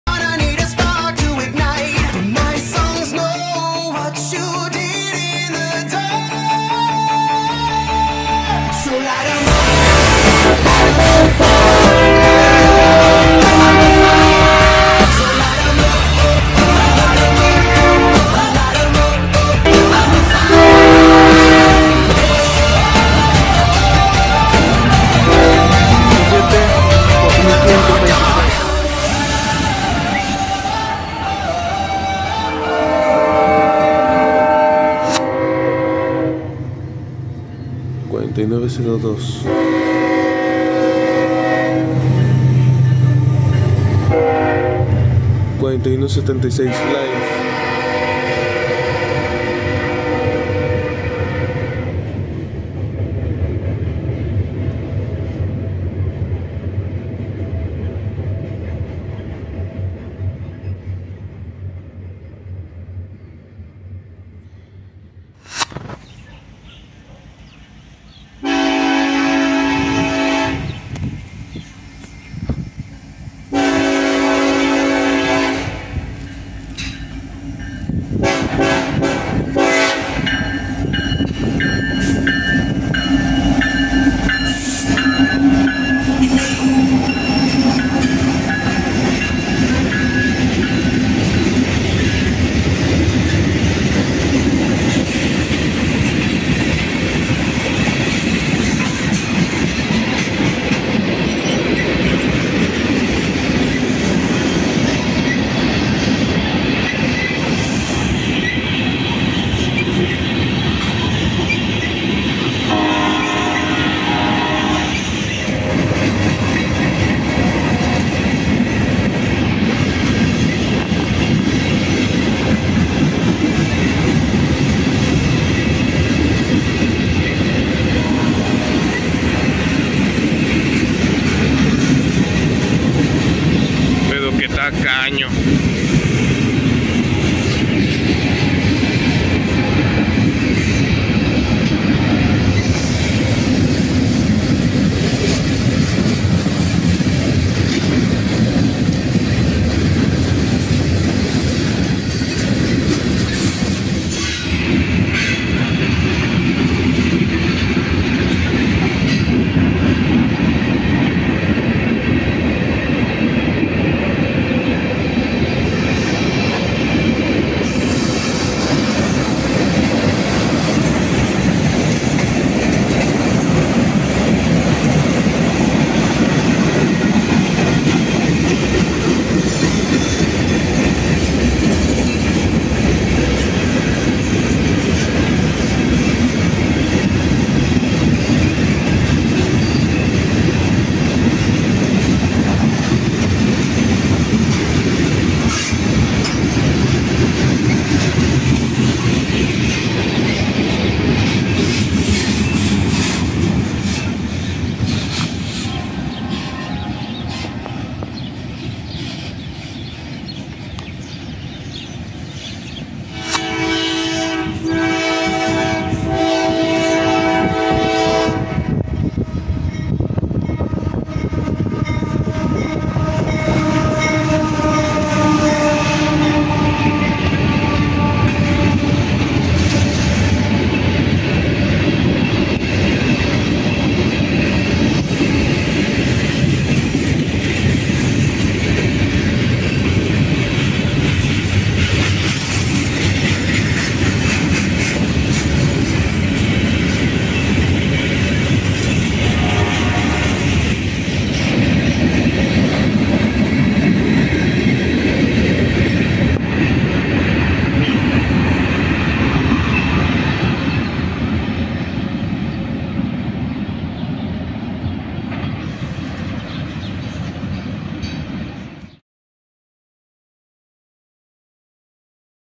Trenes KCSM y KCS en Reynosa 🤠🤠 "KCS 4586 & 4608 🙃🙃" Camión de volteó pegado en los vagones 😯😯
Vídeo Editado por YouCut.